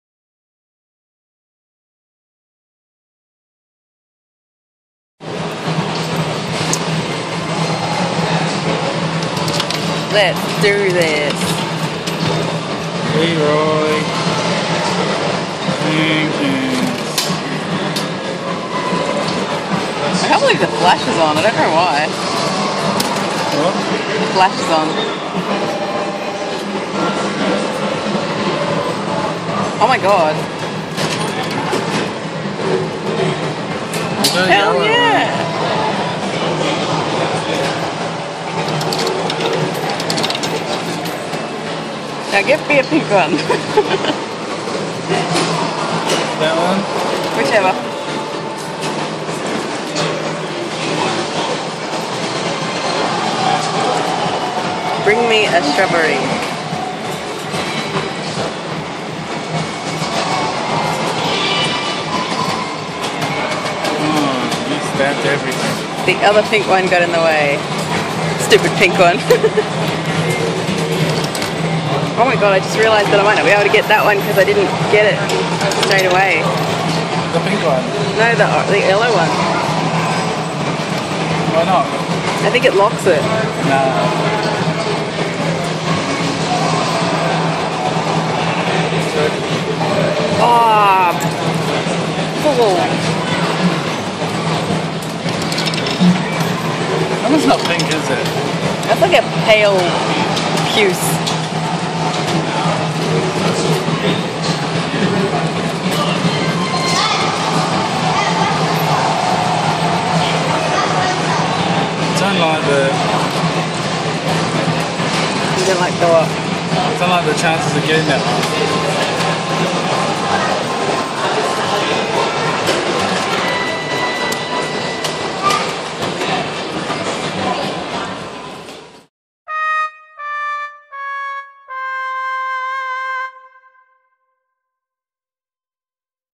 Playing Claw machine - Giant claw, yellow dragons
Was out with Courtney at Leisure Island in Perth and though we would have a crack at the giant claw.  Not a terribly lucky day but hey we got something.